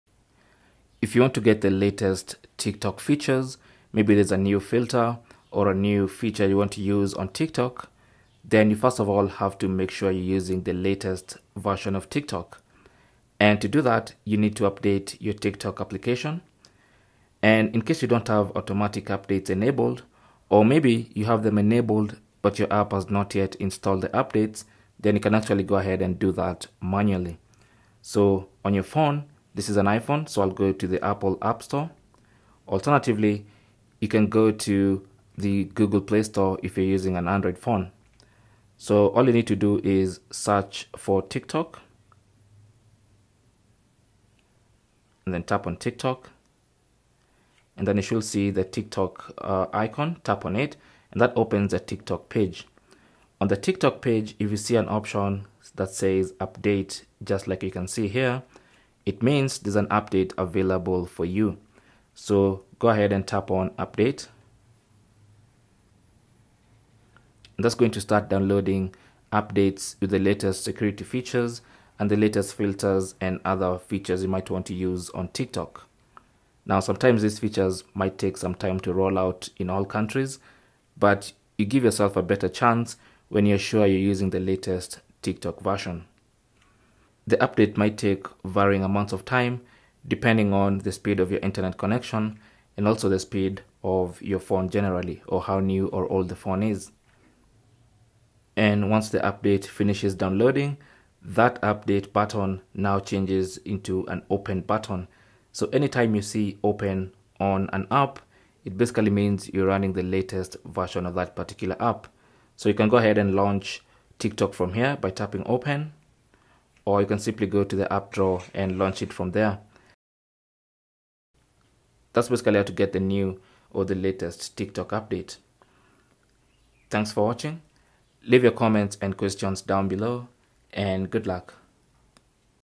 How To Get The New TikTok Update | Update TikTok App
Here's how to update TikTok.
Updates are crucial to make the apps run smoothly on your phone. With the latest features that are released, you need to update the app to enjoy them. This video will guide you on how to update TikTok.
We are using the iPhone 14 for this tutorial, but the steps are similar to those who use Android devices.

Cell Phone Tripod Adapter